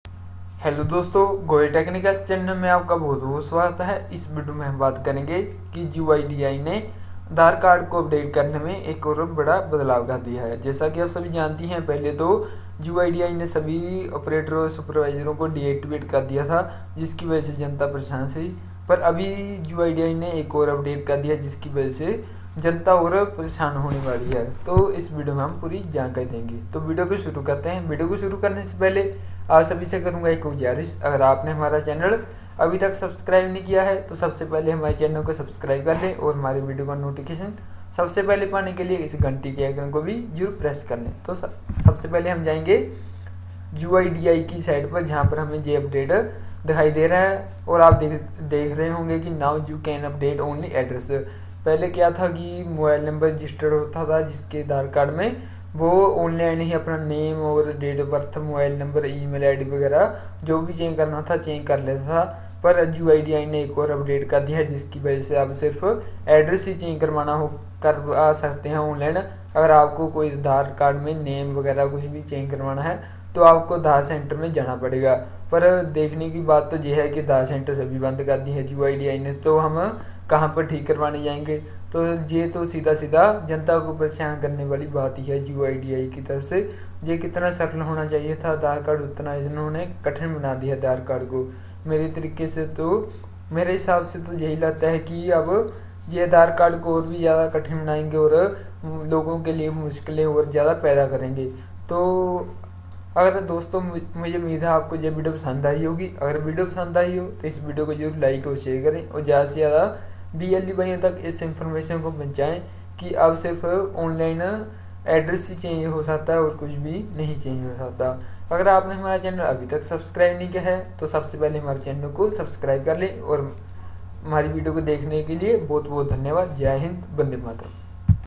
UIDAI का कहर जारी ।। UIDAI ने OPERATOR SUPERVISOR के साथ आम जनता की भी परेशानी बढ़ाई ।।
Hello dosto meine iss video mein aapko UIDAI ke bade update ke baare mein batiya hai. Muje umeed hai ki aapko yeh video jaroor pasand aayegi. Agar aapko yeh video pasand aaye to iss video ko jaroor like or share kare.

goyal technicals
goyaltechnicals